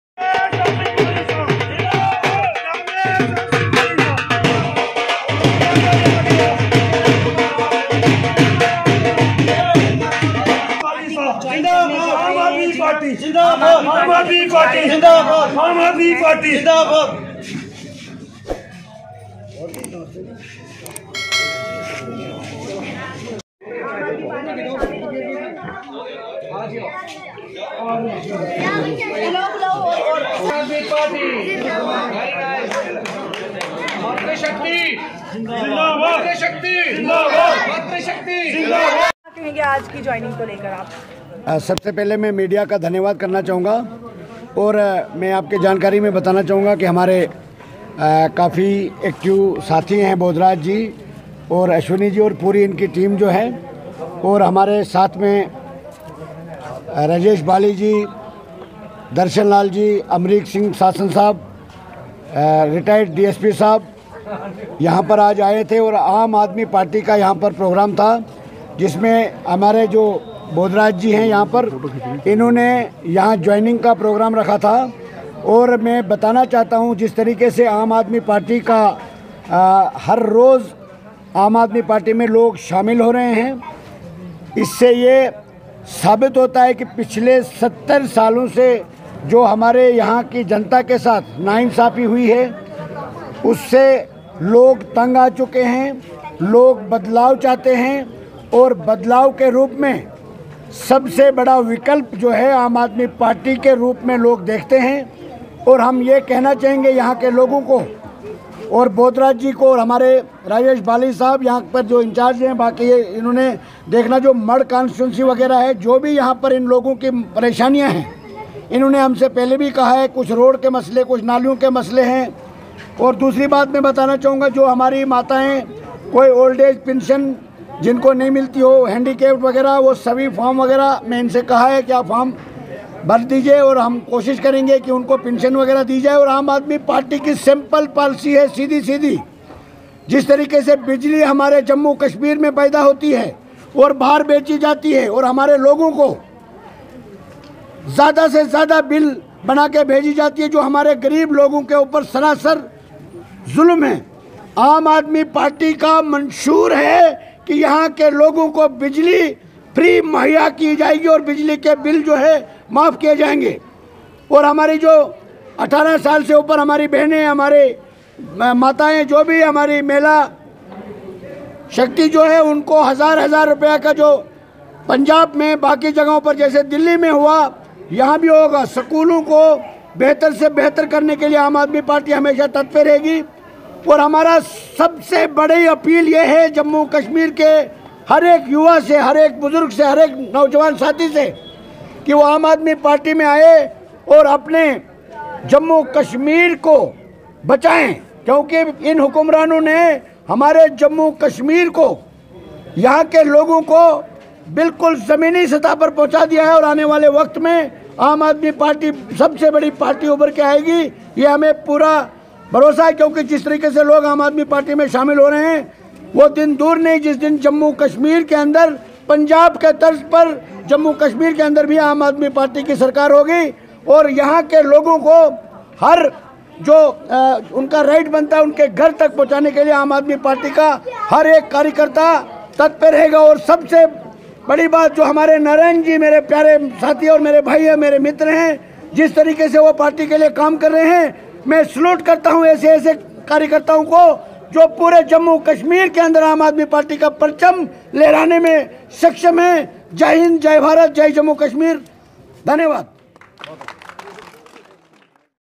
SEVERAL PROMINENT FACES JOIN AAM AADMI PARTY AT UDEHWALA IN PRESENCE OF SR. LEADER SEWA SINGH BALI
Several prominent faces in J&K  joined AAM AADMI PARTY at Udehwala Jammu in the presence of Mr. Sewa Singh Bali.

Video Journalist: Ashok Anand